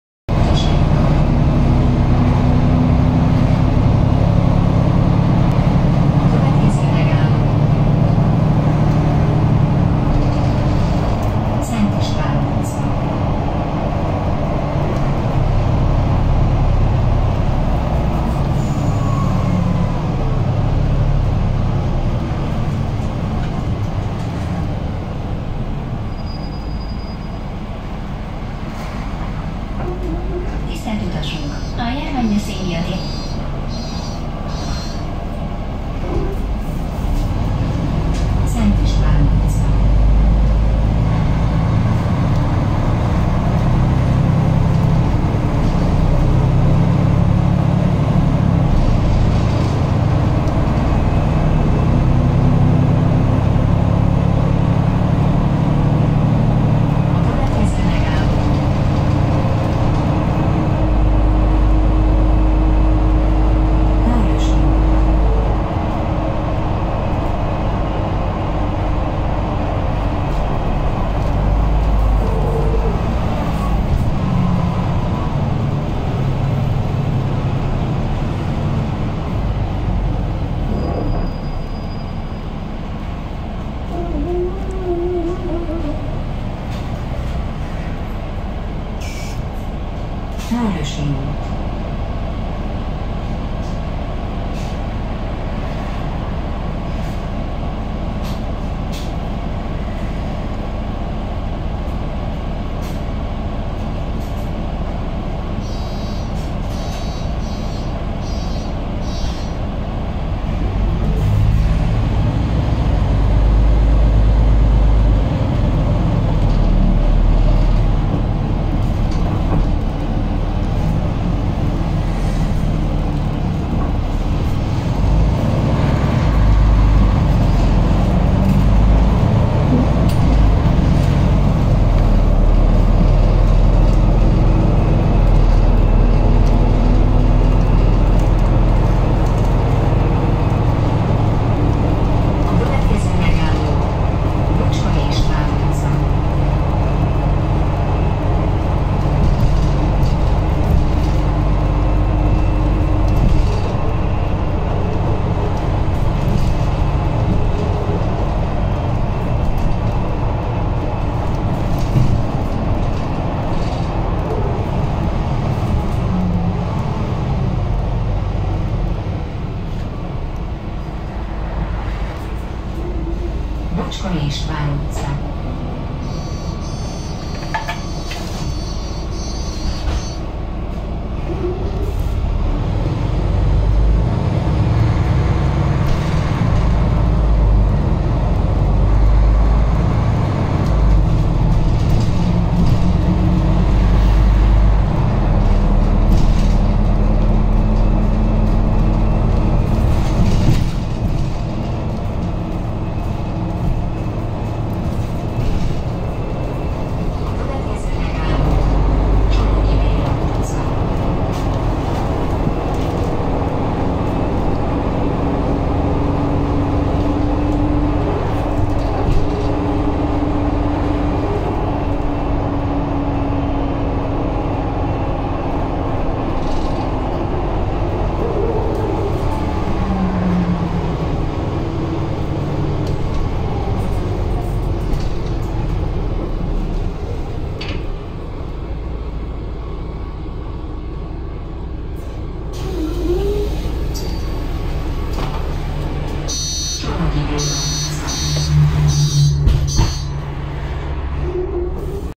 Utazás NextGen Mercedes Conecto G-vel(RVY-750)a 294E járaton Gyálon. (2020. November 14.)
Szeptemberben álltak forgalomba Budapest legújabb buszai amelyek az agglomerációban közlekednek a Volánbusz berkeiben és a Volvo 7900A-k nagy részét váltják ki. Az RVY-750 a legutolsó forgalomba állított példány nagyjából 2 hete állt forgalomba. A videó Szent István utca-Somogyi Béla utca között készült a mai nap.